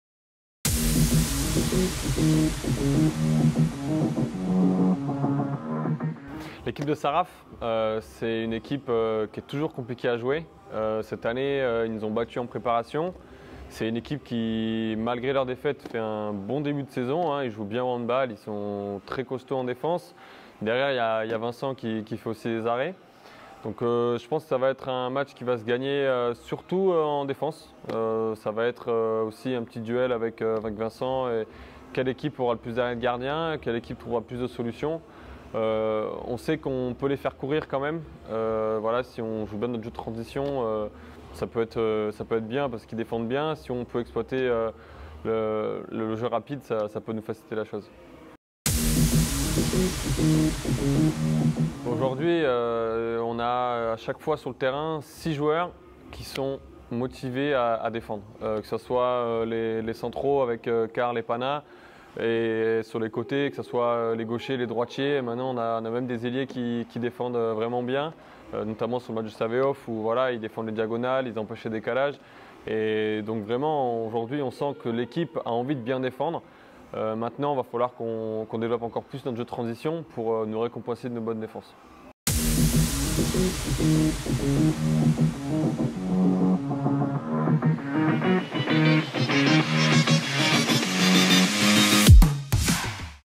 🎥 L'ITV D'AVANT MATCH - MHB vs ST RAPHAEL avec Charles BOLZINGER 🔥
Joueur incontournable de ce début de saison, Charles partage son regard sur cette équipe de Saint Raphael. Et s'attend à un duel à distance avec son alter égo Vincent Gérard.
Quand on l'interroge sur ses bonnes performances de début de saison, il rappelle avec humilité qu'il s'agit du "résultat visible" d'une équipe qui défend ensemble. il nous nous en dis plus sur la défense du MHB en ce début de saison.